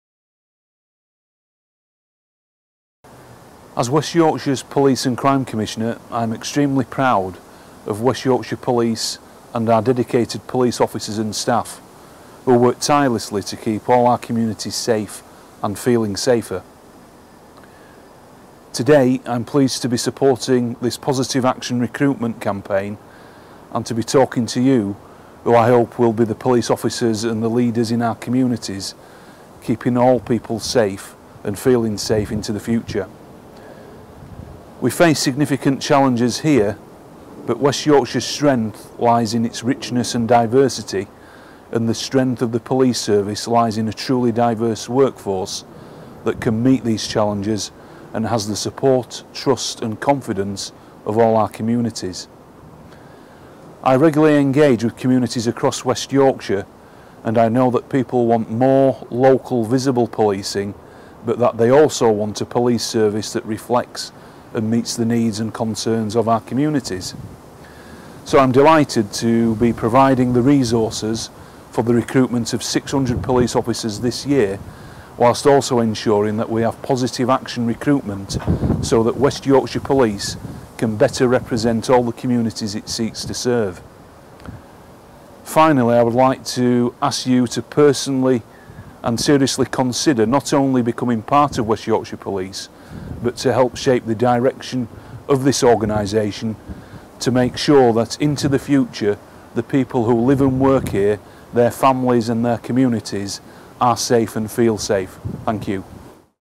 Positive Action – Dispelling the Myths: Police and Crime Commissioner Mark Burns-Williamson
Positive action is not about giving people unfair advantages, lowering standards or ticking boxes – it’s about trying to attract the very best people to join us, and making us representative of our communities. Positive action is not the ‘latest fad’ - it’s here for the long term.

Transcript:
As West Yorkshire's Police and Crime Commissioner, I am extremely proud of West Yorkshire Police and of our dedicated police officers and staff, who work tirelessly to keep all our communities safe and feeling safer.

Today, I am pleased to be supporting this positive action recruitment campaign and to be talking to you, who I hope will be the police officers and the leaders in our communities keeping all people safe and feeling safe into the future.

We face significant challenges here, but West Yorkshire's strength lies in its richness and diversity and the strength of the police service lies in a truly diverse workforce that can meet these challenges and has the support, trust and confidence of all our communities.

I regularly engage with communities across West Yorkshire and I know that people want more, local visible policing but that they also want a police service that reflects  and meets the needs and concerns of our communities.

So I am delighted to be providing the resources for the recruitment of 600 police officers this year whilst also ensuring that we have positive action recruitment so that West Yorkshire Police can better represent all the communities it seeks to serve.

I would like to ask you personally to seriously consider not only becoming part of West Yorkshire Police but to help shape the direction of this organisation to make sure that into the future the people who live and work here, their families and their communities, are safe and feel safe.